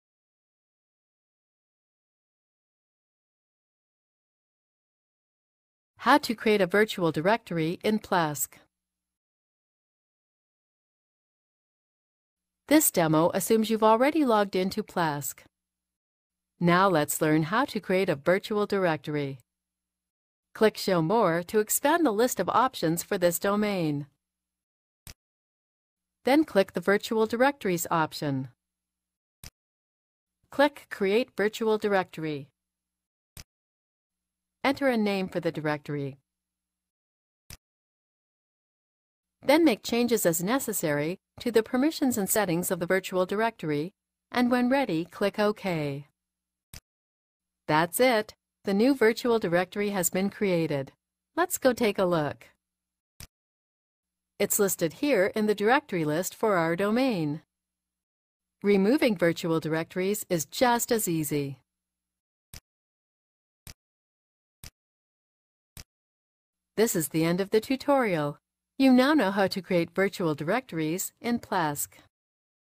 Plesk: How to Create a Virtual Directory
In this HostPapa tutorial, we show you how to create a virtual directory in Plesk.

Plesk is a commercial web hosting platform that allows server administrators to set up new websites, reseller accounts, e-mail accounts, and DNS entries through a web-based interface.